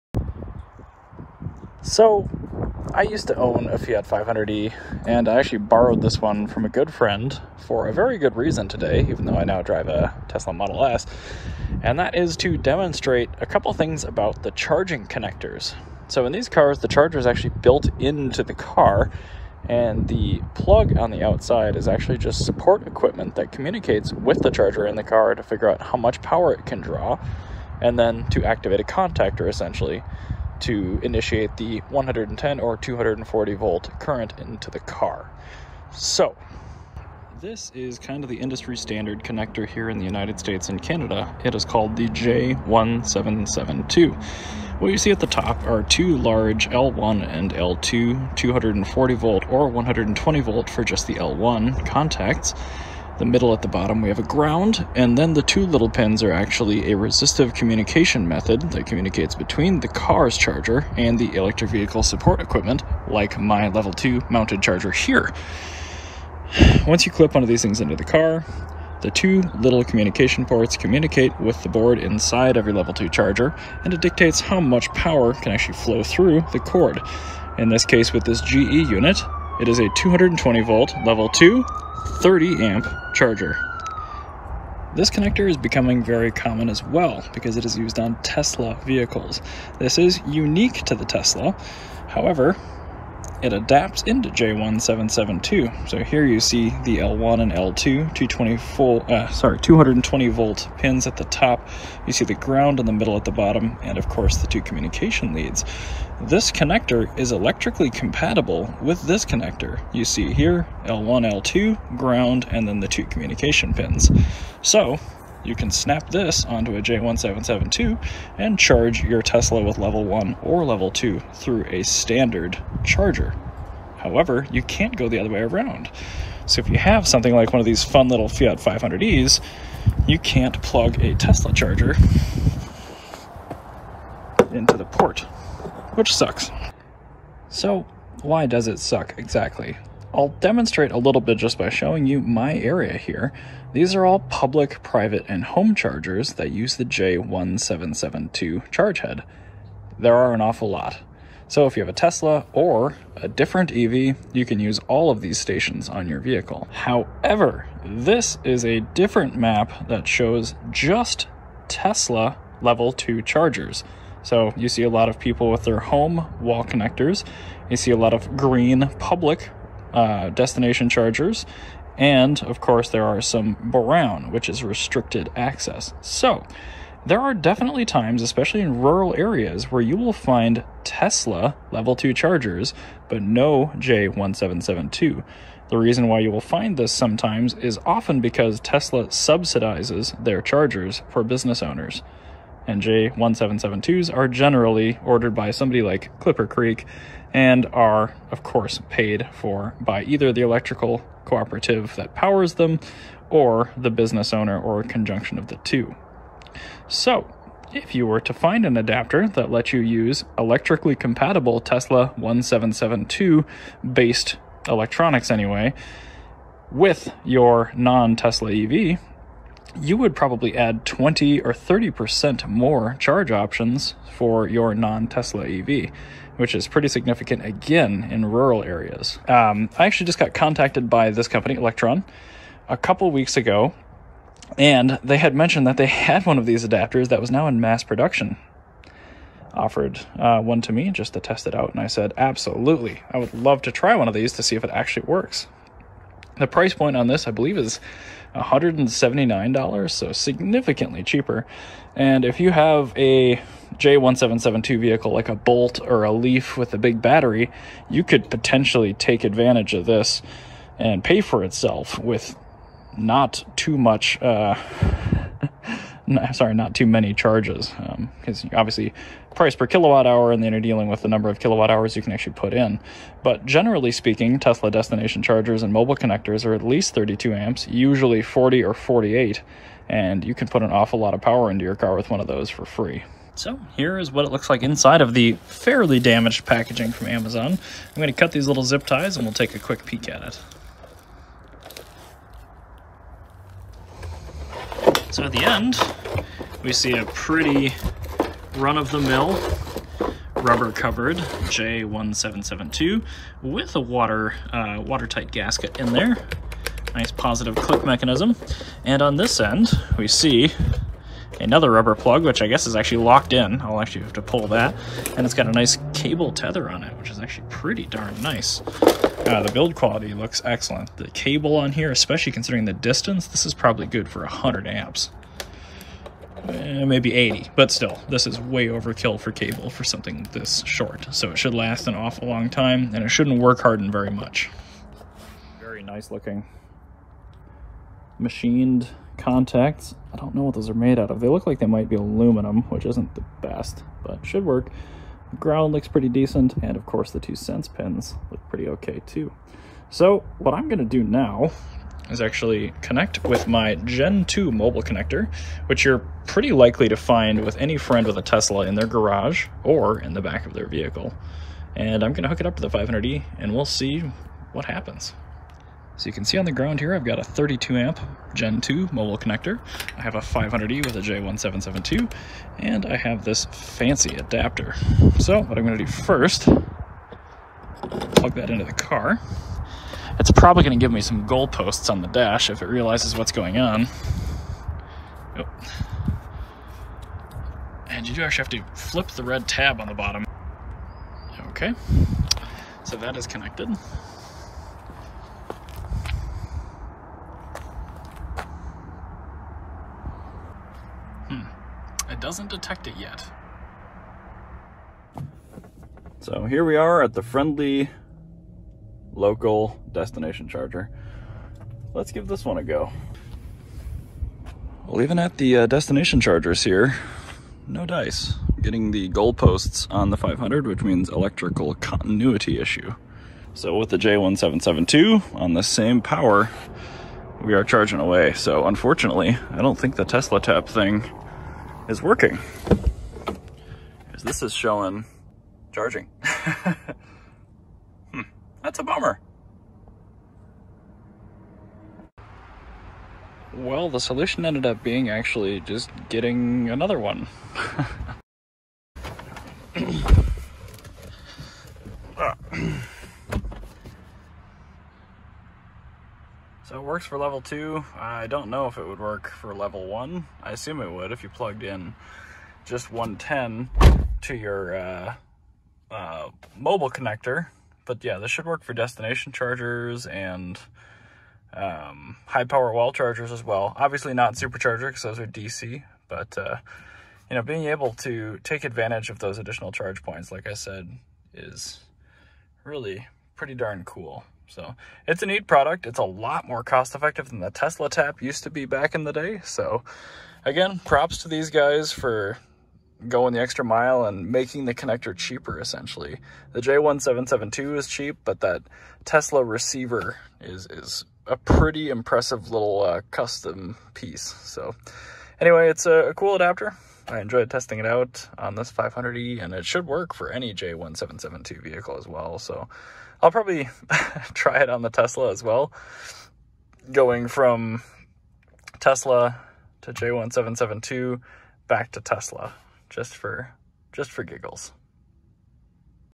Lectron J1772 Tesla charger adapter for EVs and PHEVs
This adapter allows your J1772 standard equipped BEV or PHEV to use any Tesla wall connector, mobile connector, destination charger or HPWC!

Add 30% more free coptions for charging to your trips with this one adapter.

2013 Fiat 500e